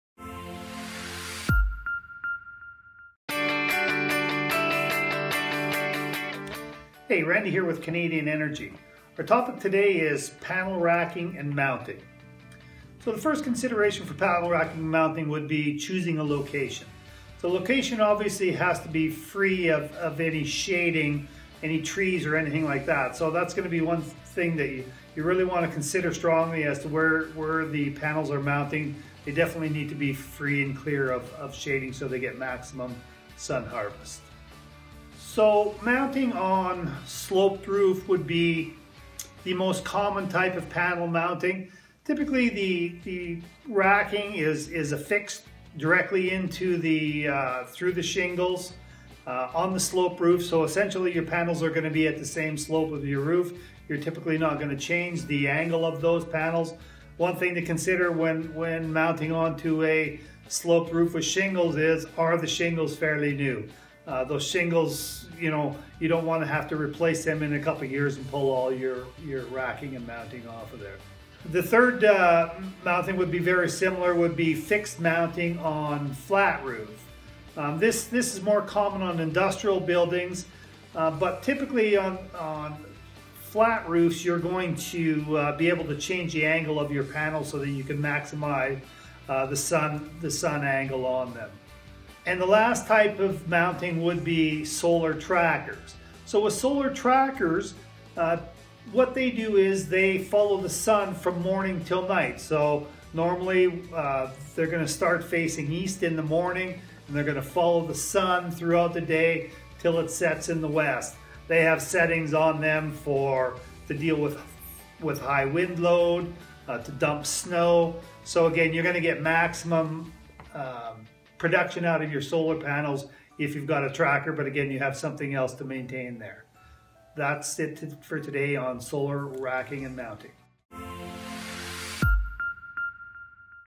Canadian Energy™ - Living Off-Grid 101 - Panel Mounting and Racking (6/12)
Maximizing sun harvest is the most important consideration when it comes to solar panels. You have to think about where you'll put them, what direction they'll face, and what angle will allow for the most sun exposure. We discuss these details and different types of panel mounts in this video on panel mounting and racking.